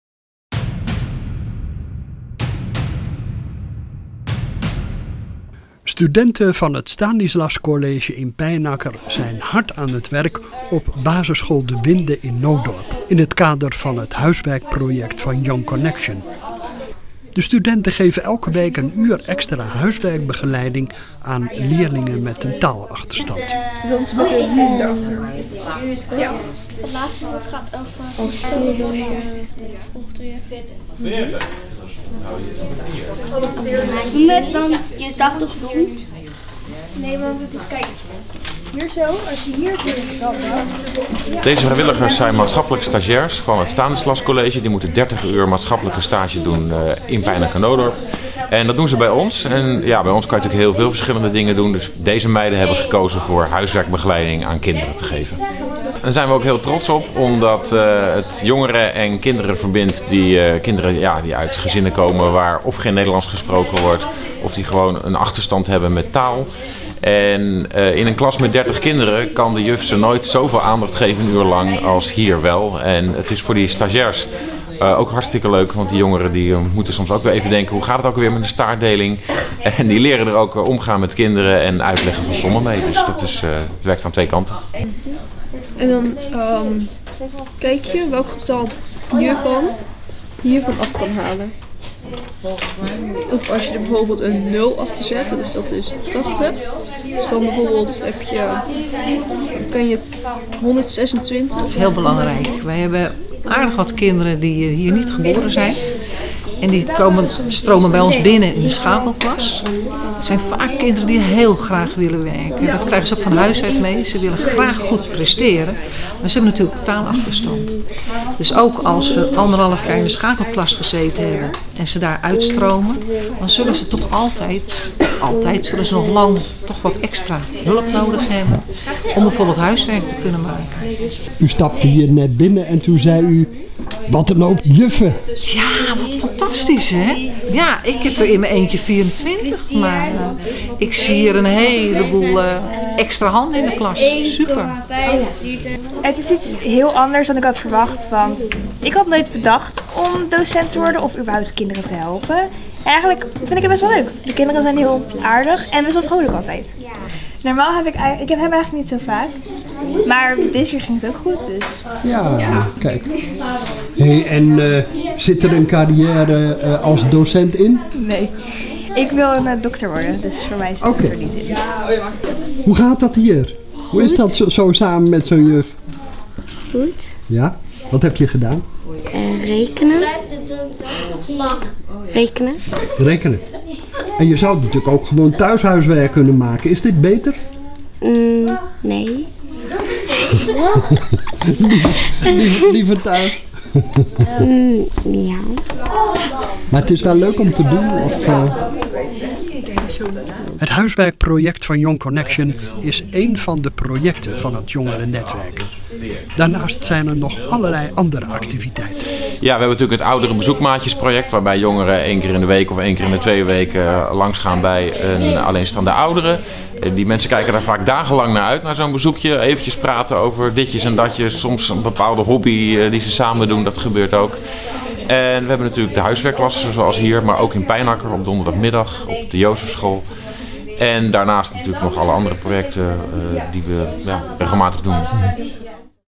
Huiswerkproject Young Connection
Studenten van het Stanislascollege in Pijnacker zijn hard aan het werk op basisschool De Winde in Nootdorp, in het kader van het huiswerkproject van Young Connection. De studenten geven elke week een uur extra huiswerkbegeleiding aan leerlingen met een taalachterstand.